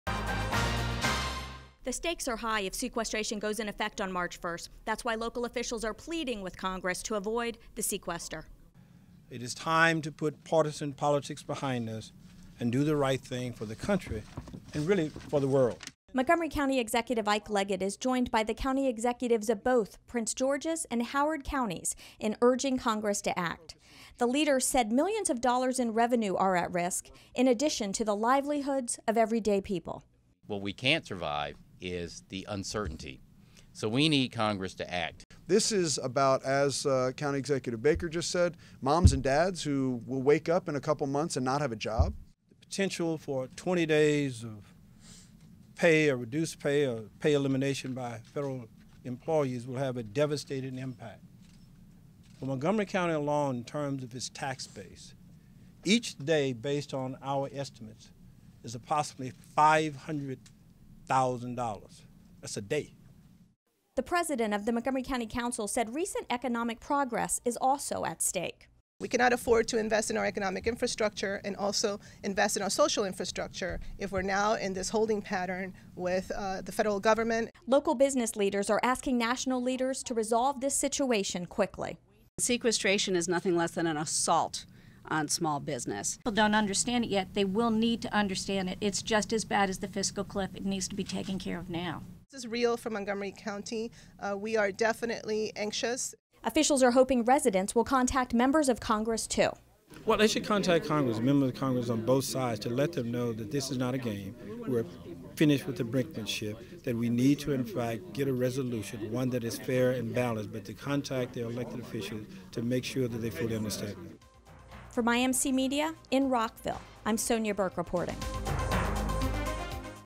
Area County Executives and Business Leaders Urge Congress to Avoid Sequester
Montgomery County Executive Isiah Leggett, Prince Georges County Executive Rushern Baker and Howard County Executive Ken Ulman are urging Congress to find a way to avoid the sequester.

The three county executives held a news conference on Feb. 19 at the Executive Office Building in Rockville. They were joined by several representatives from area chambers of commerce and Montgomery County Council President Nancy Navarro